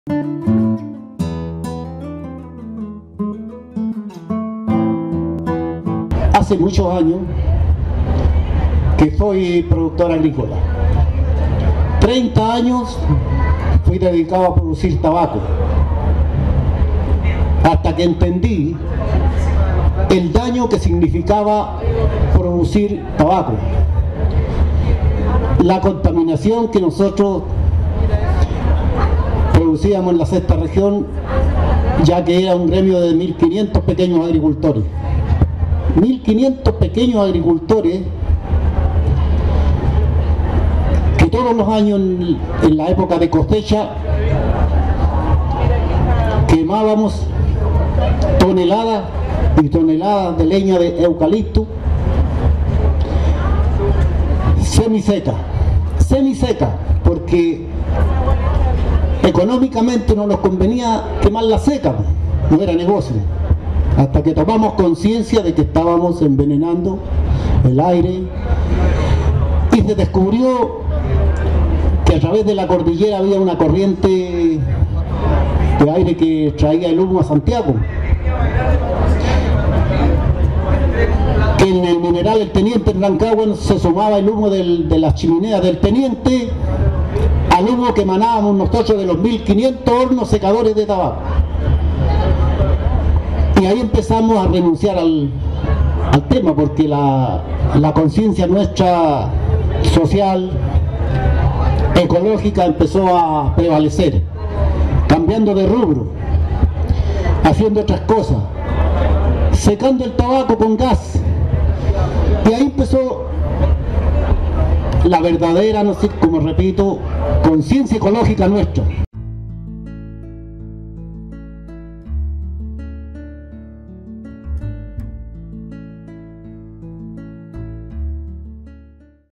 La CONCIENCIA ECOLOGICA 👩🏽‍🌾 campesina chilena
Quielo Rivera, dirigente campesino, nos comenta sobre cómo nació la conciencia 👩🏽‍🌾 ecológica de ellos en cuanto al cuidado medioambiental

Este es el extracto de una charla titulada "Conversatorio sobre Agricultura Ecológica con INDAP", que se encuentra disponible en

👇PARA APRENDER MÁS 🔴✍🛒📽📈📊🎁🔴

🙂Cualquier duda nos consultas 🙌

Texto: guitarclas Extracción texto del video -
Video: guitarclas -